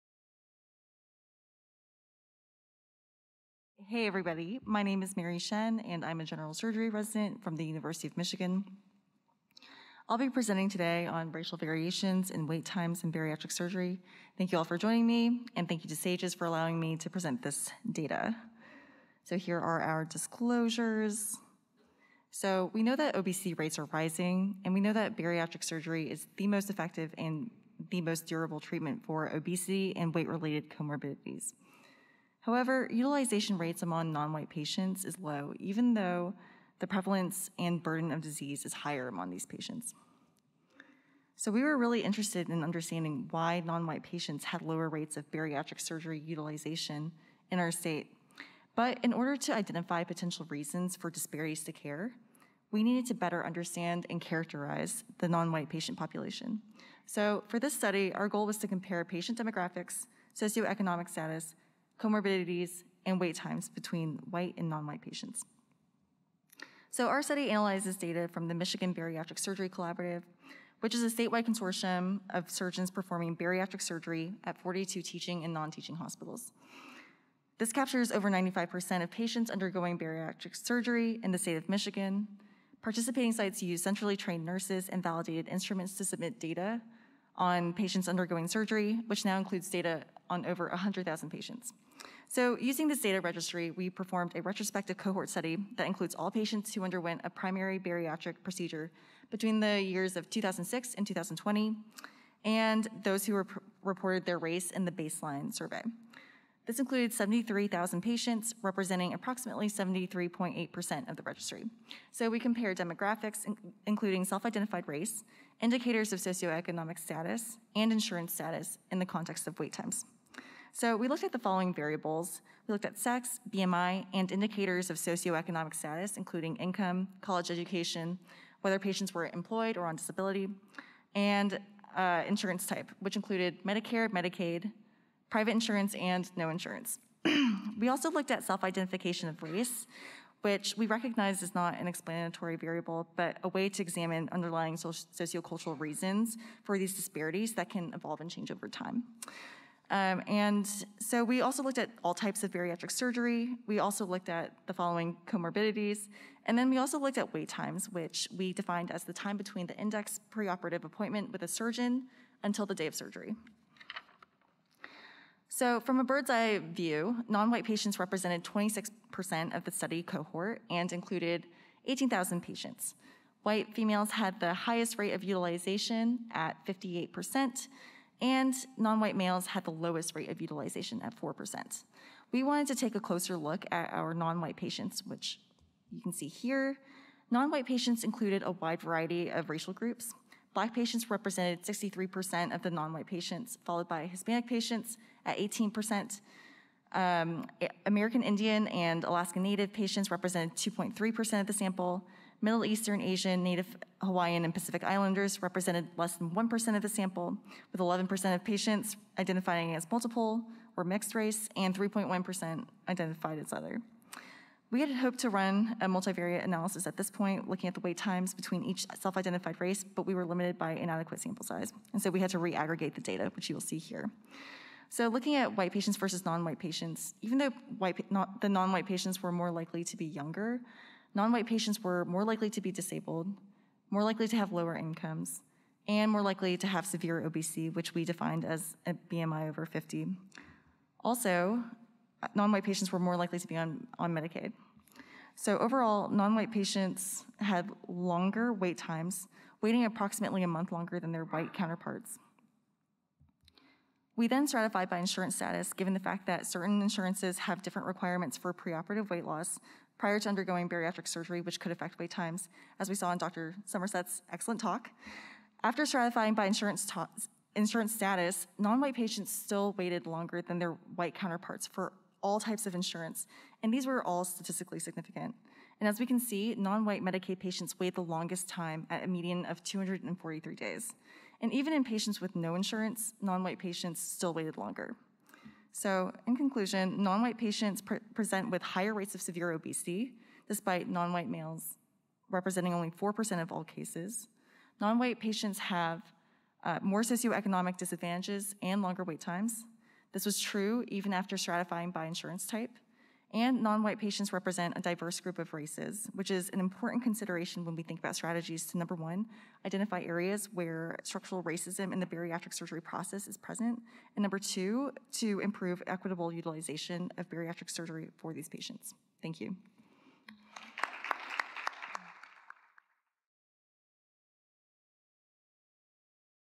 Racial Variation in Baseline Characteristics and Wait Times Among Patients Undergoing Bariatric ...
This talk was presented by Mary Shen, MD, MS during the “SS27: Bariatric Session 2” session at the SAGES 2021 Annual Meeting on September 2, 2021. This talk was selected for presentation using a blinded peer-review process.

Racial Variation in Baseline Characteristics and Wait Times Among Patients Undergoing Bariatric Surgery: Not as Simple as Black and White